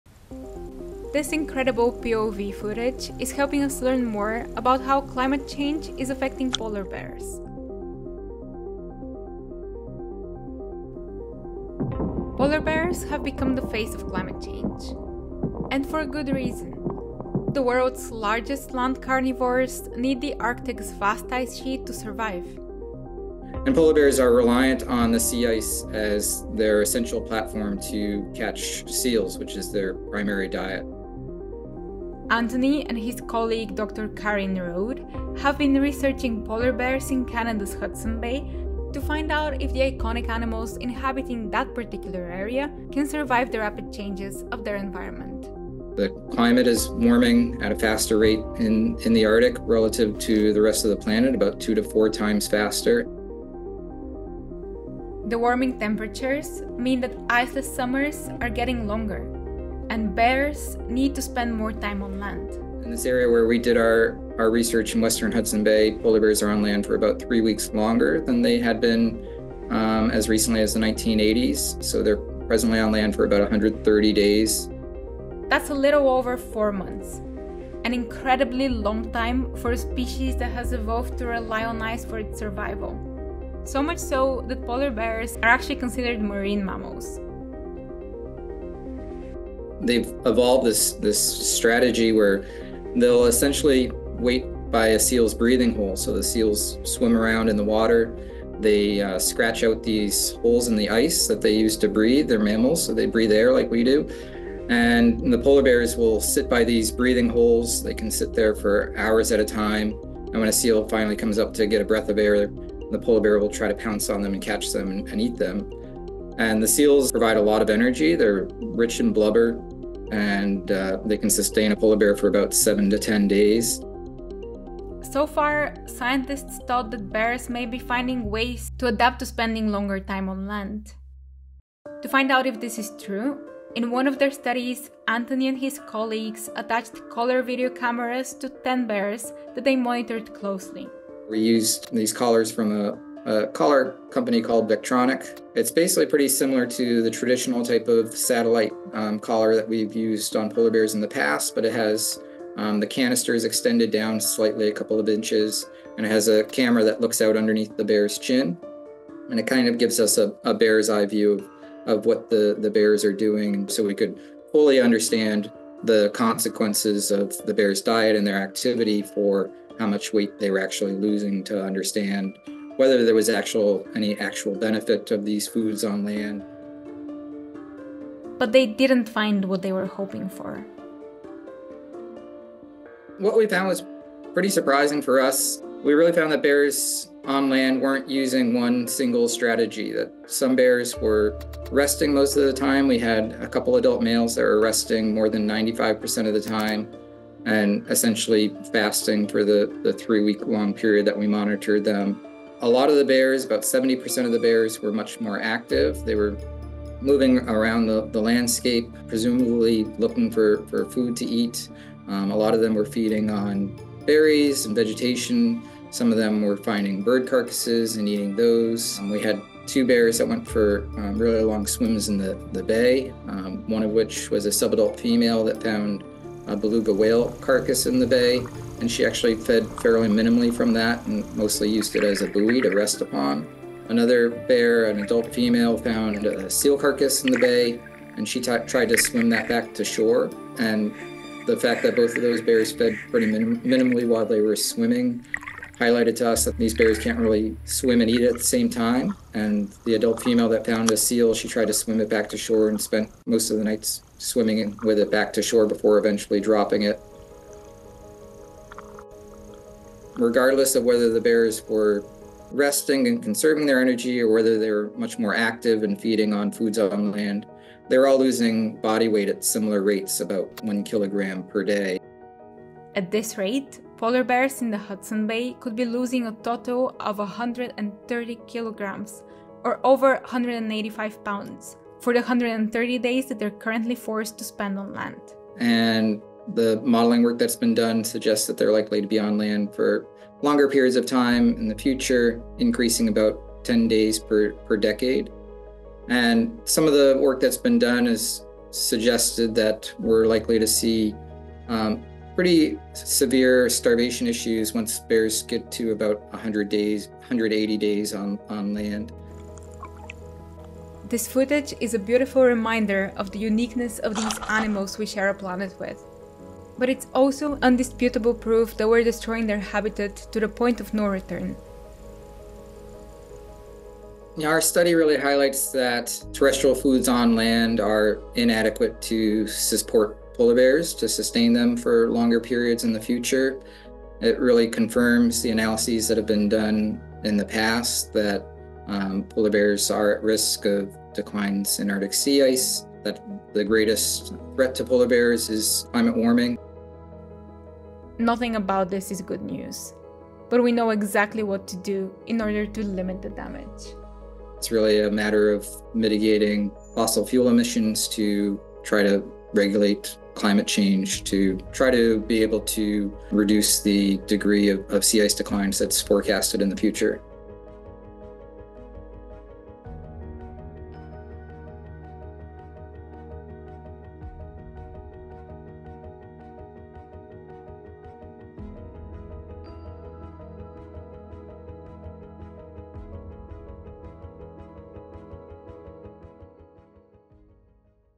Rare POV footage captures polar bears in their melting habitat
This incredible POV footage, captured in Canada's Hudson Bay, is helping us learn more about how climate change is affecting polar bears.

The climate in the Arctic is warming about two times faster than the rest of the planet. As a result, polar bears in the Hudson Bay are forced to spend more time on land, recently reaching 130 days of terrestrial life – a three-week increase since the 1980s.

So far, scientists have suspected that the iconic marine mammals may be able to adapt to iceless life. New research that used collar POV cameras to track the bears' behavioural changes on land, however, portrays a different reality. In this video, Mashable spoke to Dr. Anthony Pagano, a Research Wildlife Biologist and co-author of the study, about the results of their findings.